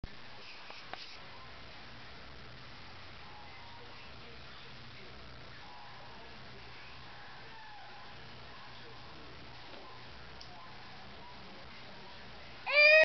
Rock is sleepy
Rock falling asleep at dinner